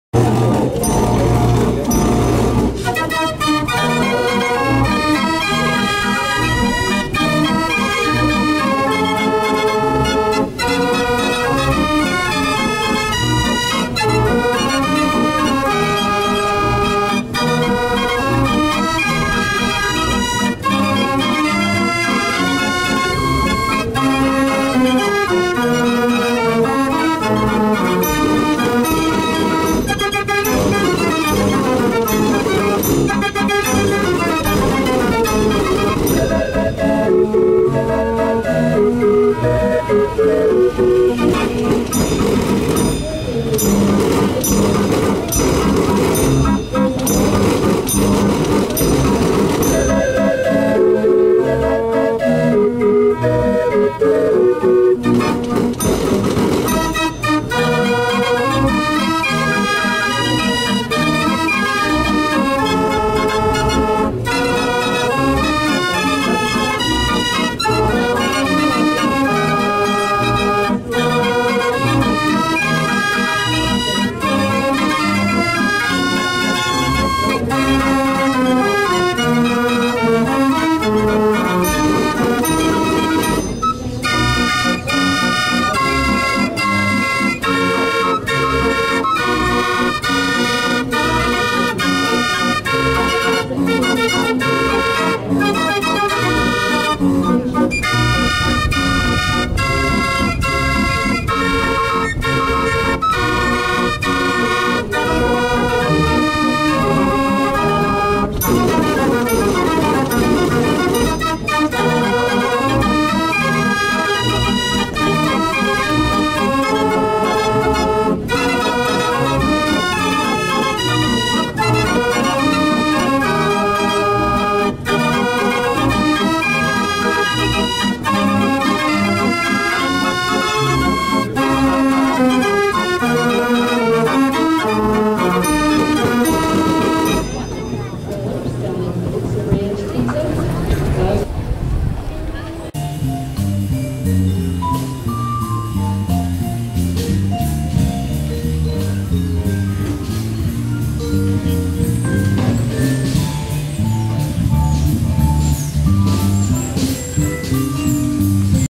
Orange City IA Tulip Festival 2012
Orange City IA: Despite the absence of tulips, the annual Tulip Festival went on to the pleasure of thousands. Although the tulips bloomed early this year, this didn't stop the many other activities from occuring, including the presentation of the restored "Dutch Street Organ", whose music you hear in the background.